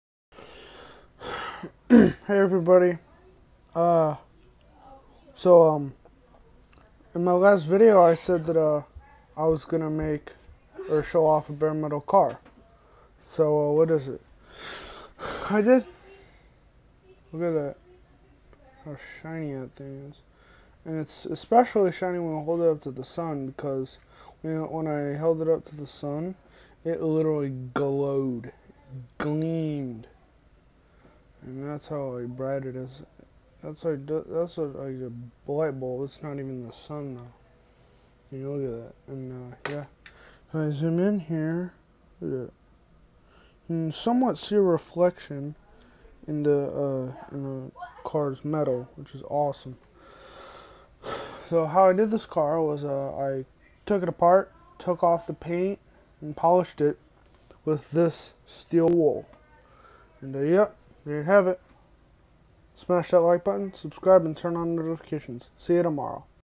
A bare metal car
So in my last video, I said I was going to show you a bare metal car I made earlier. So that’s what I did. For those of you who don’t know what a bare metal car is, it’s a car that’s been stripped of paint, and cleaned, sometimes to a mirror shine. And you know how I said I wouldn’t be posting every day after August 8th? So after August 8th, I will post once or twice every weekend. See you tomorrow